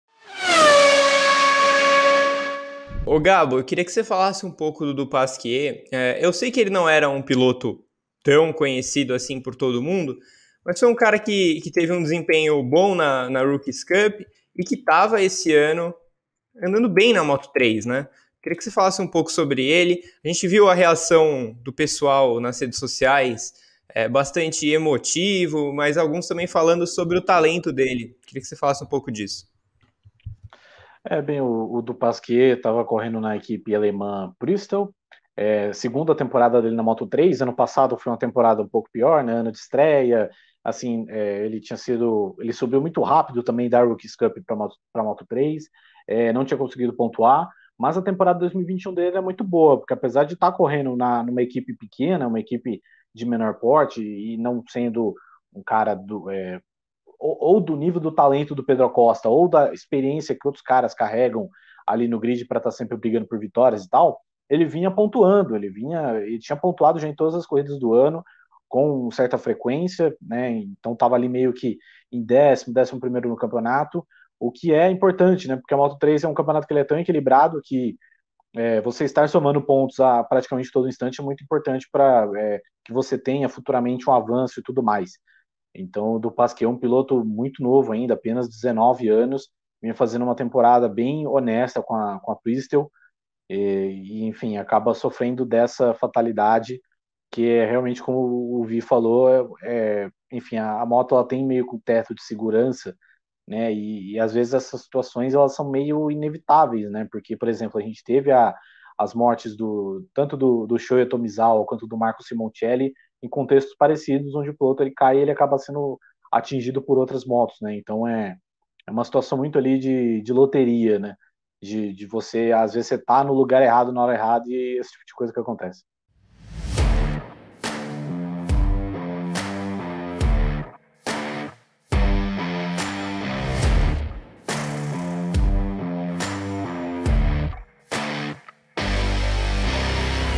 DUPASQUIER, DA MOTO3, ESTAVA 'NO LOCAL ERRADO E NA HORA ERRADA', AVALIA GABRIEL CARVALHO
No Paddock Plus, Gabriel Carvalho fala sobre como a morte de Jason Dupasquier, na Moto3, veio por estar em local errado e hora errada, tal como outros casos recentes no Mundial de Motovelocidade.